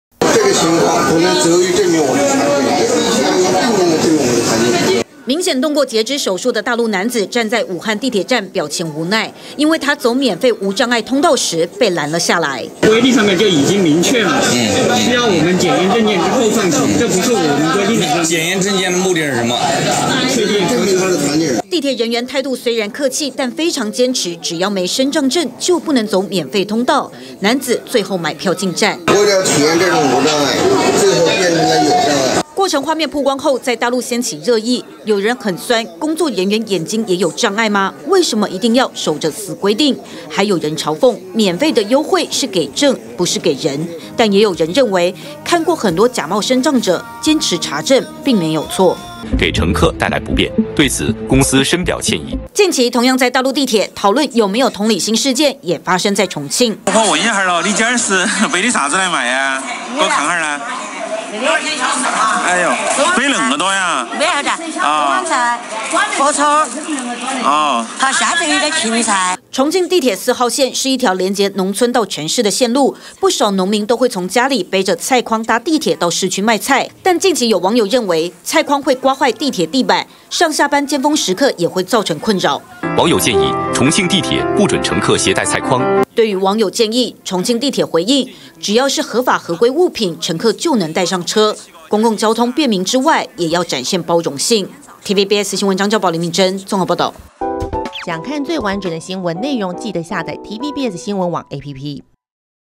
硬要截肢男出示身障證 武漢地鐵被罵翻｜TVBS新聞
大陸一名雙手截肢的男子，日前搭武漢地鐵走免費的無障礙通道時，被工作人員攔下要求出示"殘障證"。即便男子說自己因為沒雙手，不方便帶證件還是無法通行。畫面曝光後引發民怨，有人痛批地鐵只會守著死規定，管理制度僵化，沒有同理心。